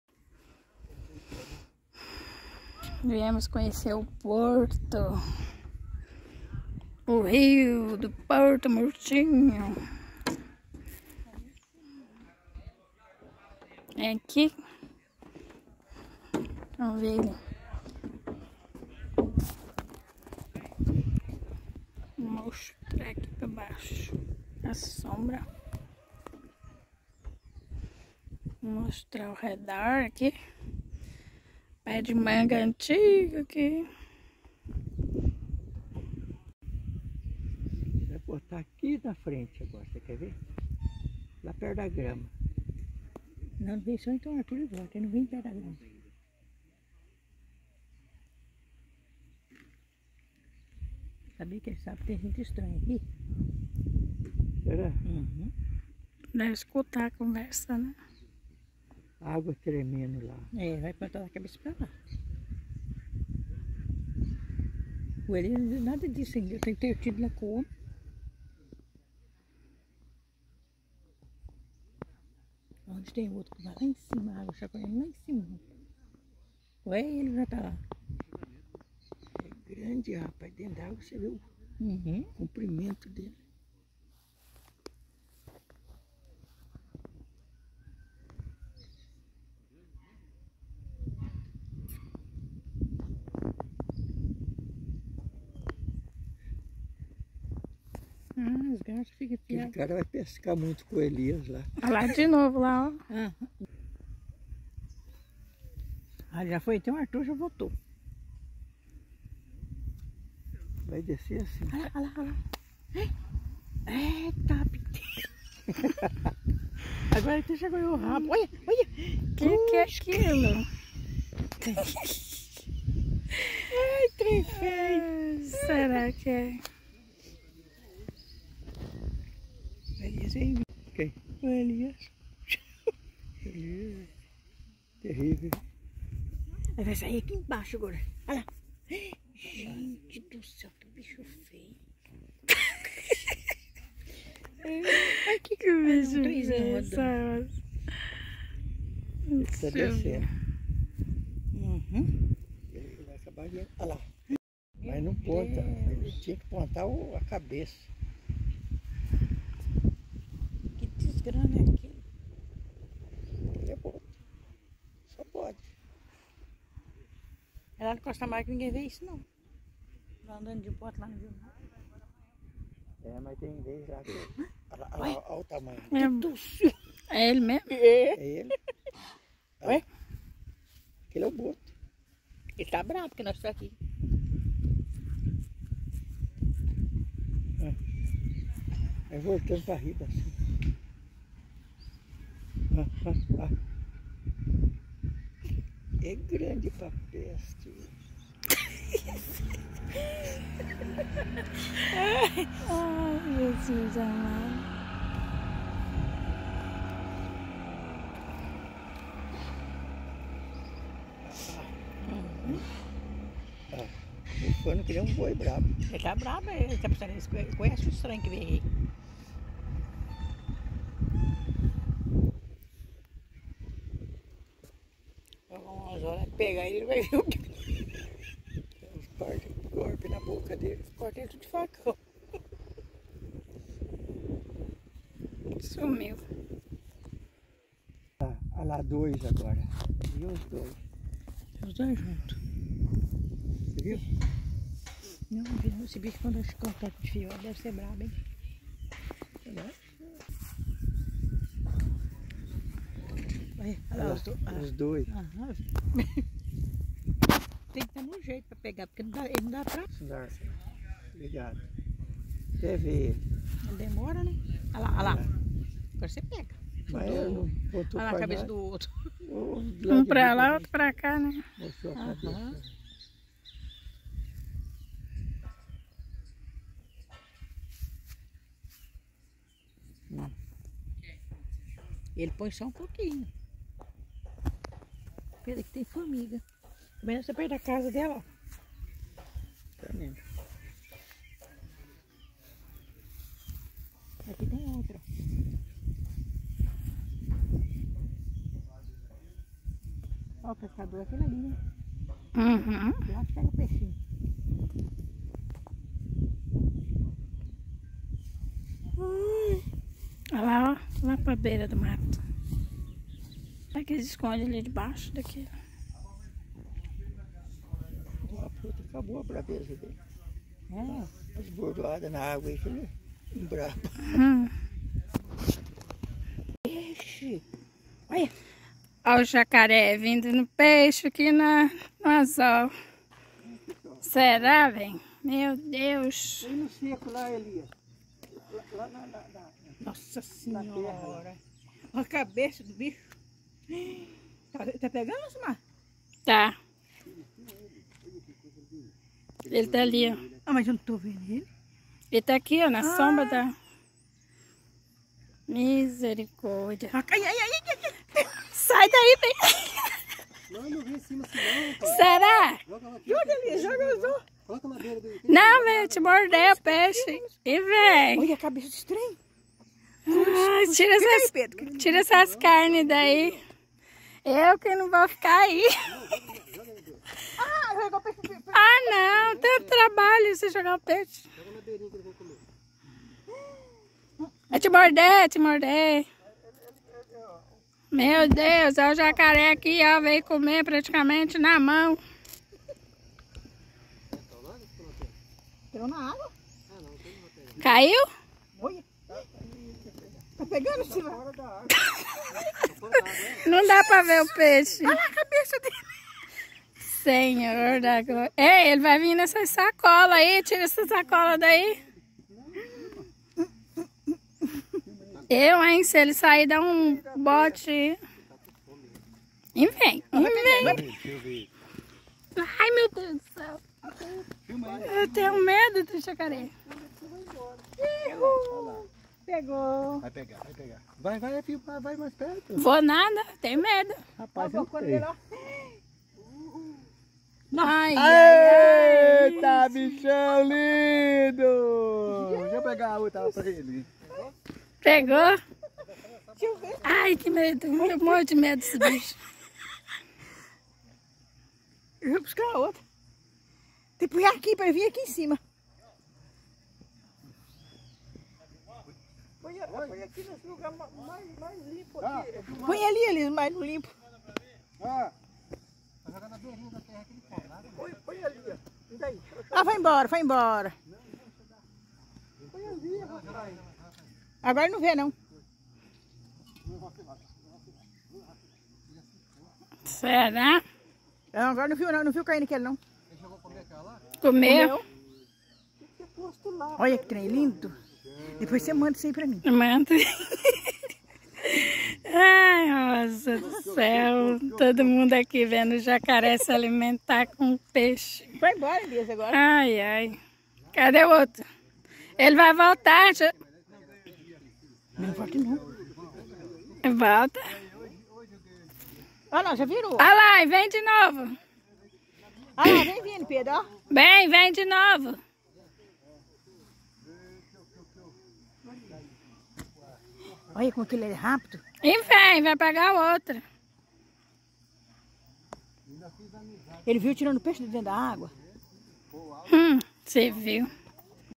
CONHECEMOS O RIO DO PORTO MORTINHO - @Canal Aprendiz
Hoje vou mostrar o dia que fomos conhecer o rio daqui da região de Porto mortinho em São Francisco do guapore Rondônia.
Coisa linda! e meu esposo deu um peixe que pesquei para um jacaré comer.